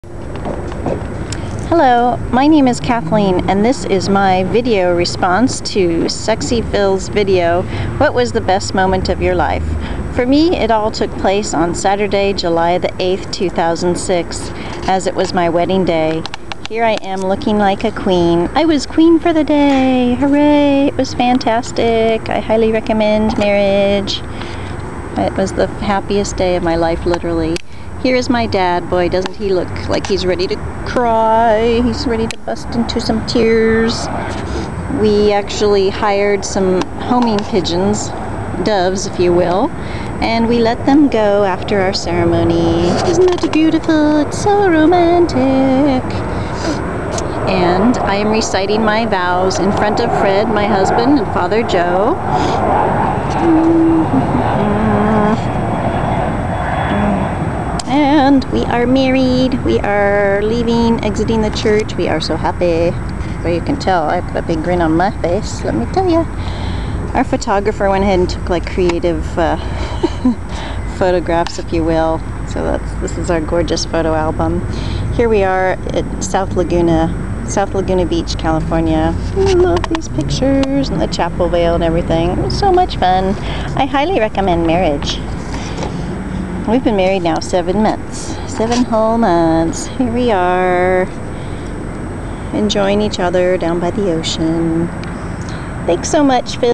Re: Most Memorable Time of my Life
I had the time of my life--Wedding day and wedding photos.  Kathleen + Fred=Saturday, July 8th, 2006.